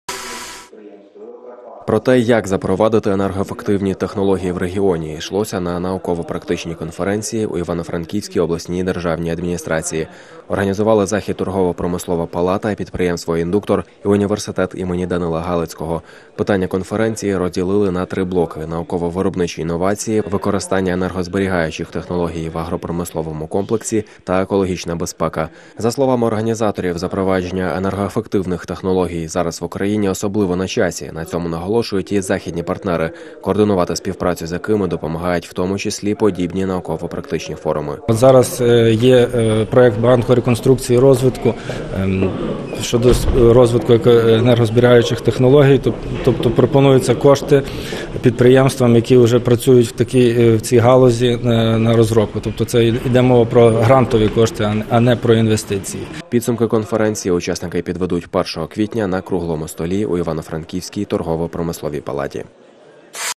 Енергоефективні технології
Про те, як запровадити енергоефективні технології в регіоні, йшлося на науково-практичній конференції в Івано-Франківській обласній державній адміністрації. Організували захід торгово-промислова палата, підприємство “Індуктор” і університет імені Данила Галицького. Питання конференції розділили на три блоки: науково-виробничі інновації, використання енергозберігаючих технологій в агропромисловому комплексі та екологічна безпека. За словами організаторів, запровадження енергоефективних технологій зараз в Україні особливо на часі. На цьому наголошують і західні партнери, координувати співпрацю з якими допомагають в тому числі подібні науково-практичні форуми.